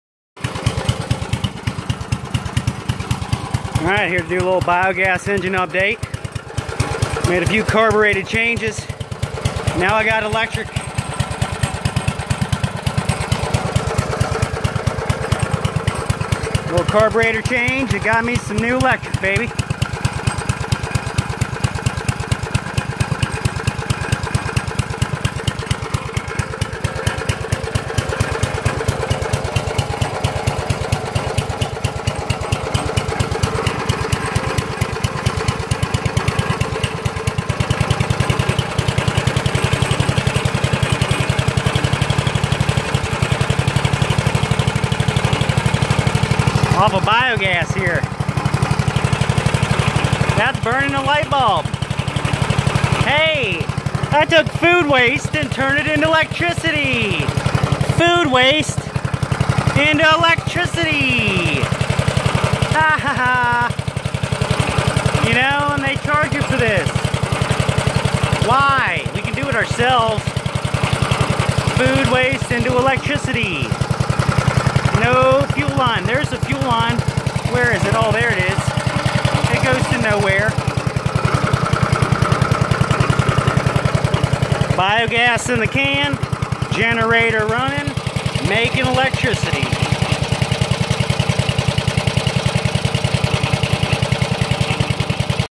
Biogas electricity
Generator biogas fuel ,free energy from food waste.